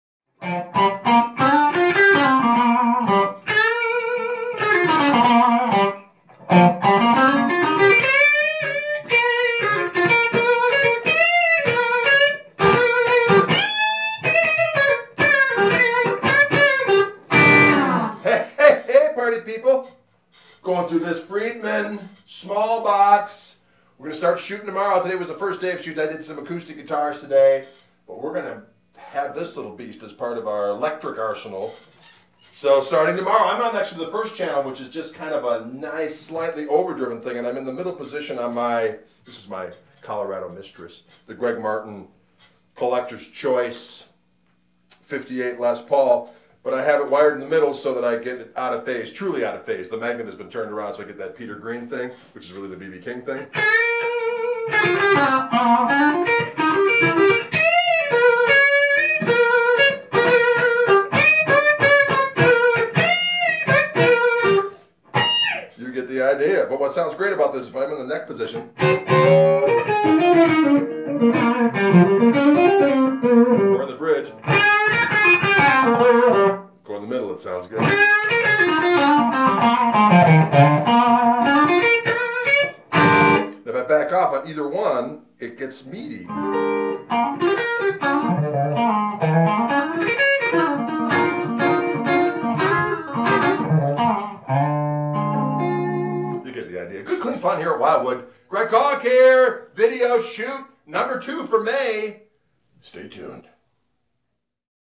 Greg Koch and His Greg Martin Les Paul
In one of our recent videos, Greg Koch is playing his Greg Martin Les Paul Collectors Choice through a Friedman Smallbox 50 amplifier head and 2X12 Cab. Many questions  were brought up regarding the potential out-of-phase middle position on the guitar. He addresses those inquiries here.